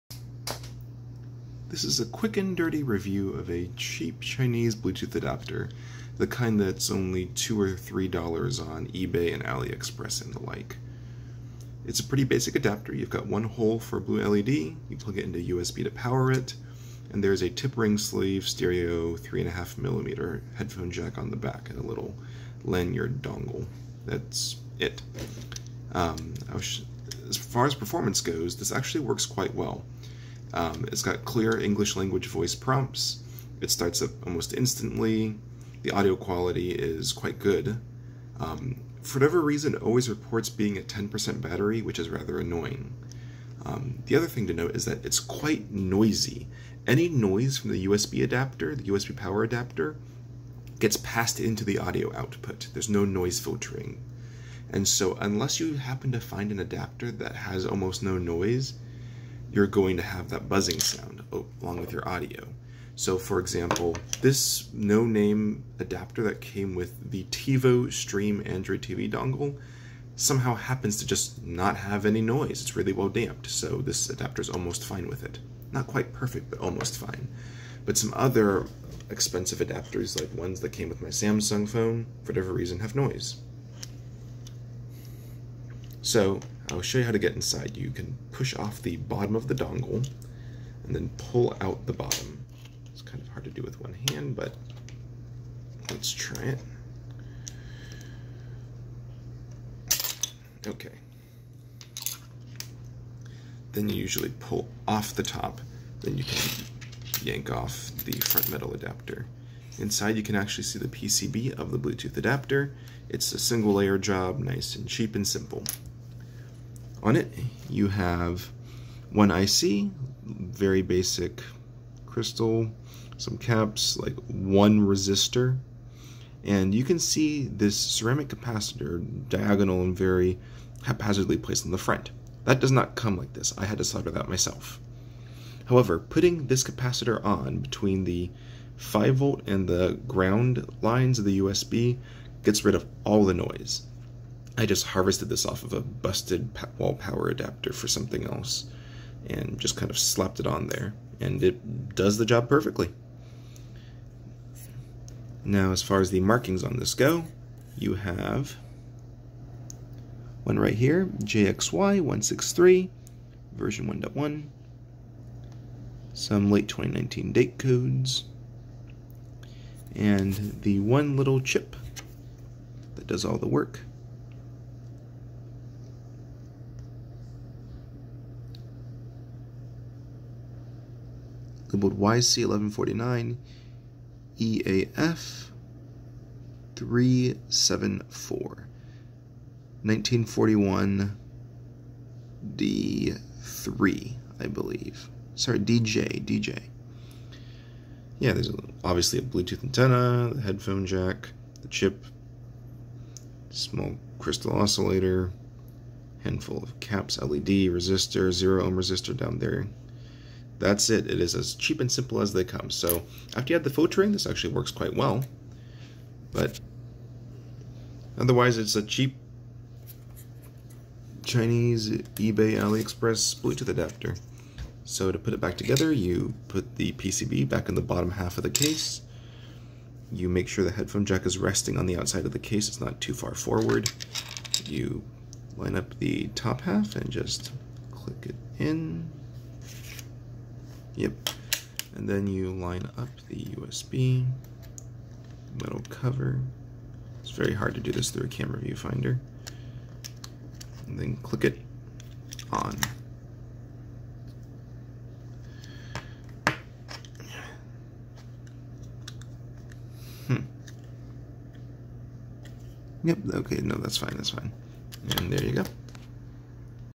Review and mod for the cheapest Bluetooth USB 3.5 mm adapter dongle
This is the cheapest US shipped Bluetooth adapter on eBay (but I'm sure it's also on Wish, AliExpress, Amazon, and other websites).  This video has a quick review of it (pretty good) and a fix for the background buzzing and noise (adding a capacitor).  The PCB is model JXY163 (dated in 2019) and the Bluetooth IC is labeled YC1149 EAF374 1941DJ.